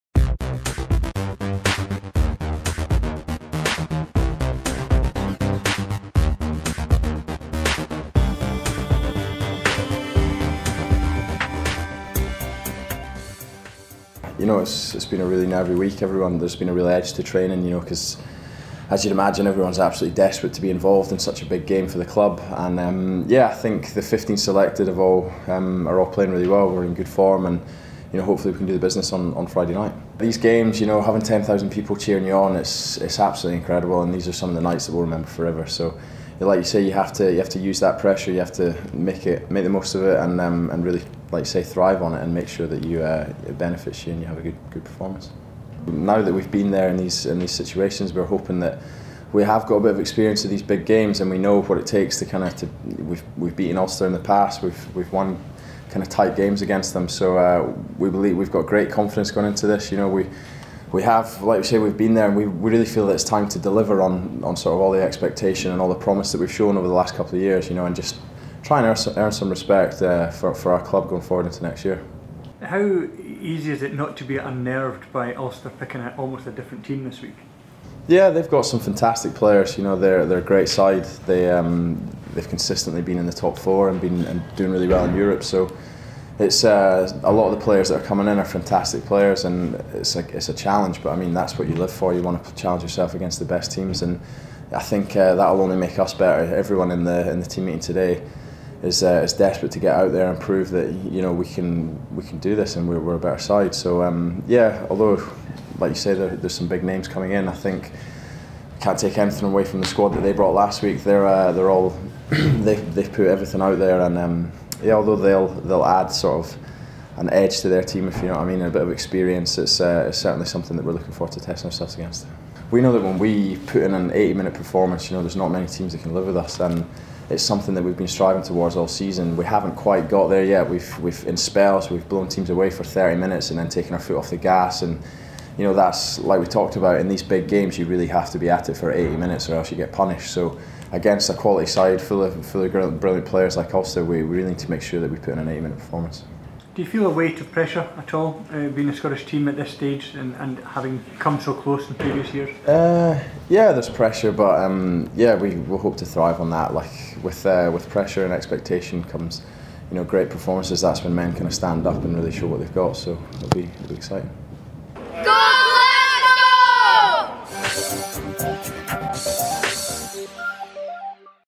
Daily Record Rugby Show - Peter Horne ready for Ulster
Glasgow and Scotland centre Peter Horne insists that Warriors are more than capable of beating Ulster and their restored star names in Guinness PRO12 semi-final at Scotstoun on Friday night